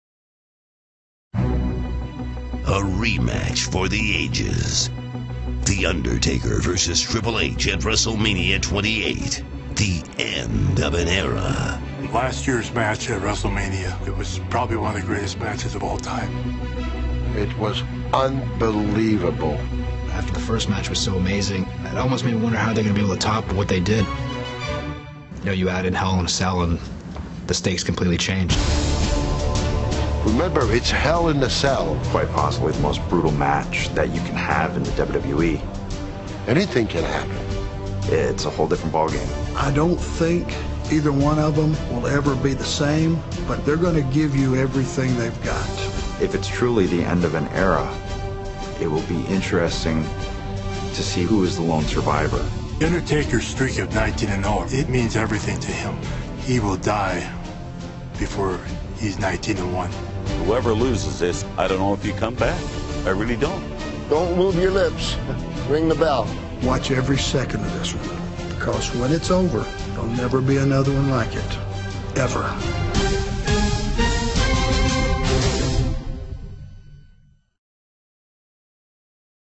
A rematch for the ages, the `End of an Era`
WWE Superstars and Legends weigh-in on the "End of an Era" match between The Undertaker and Triple H.